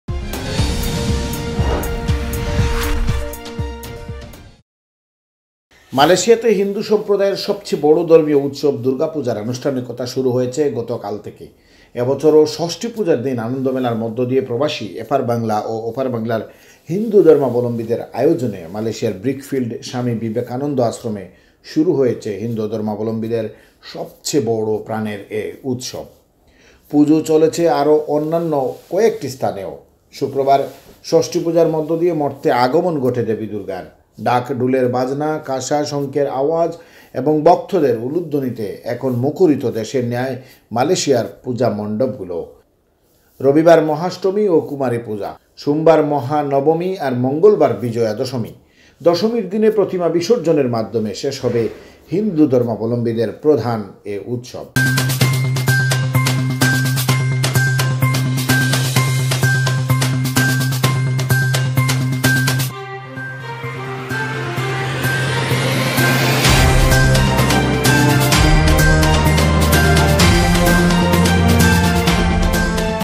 মালয়েশিয়াতে দুর্গাপূজার আনুষ্ঠানিকতা শুরু হয়েছে গতকাল থেকে।
মালয়েশিয়াতে  হিন্দু সম্প্রদায়ের সবচেয়ে বড় ধর্মীয় উৎসব দুর্গাপূজার আনুষ্ঠানিকতা শুরু হয়েছে গতকাল থেকে।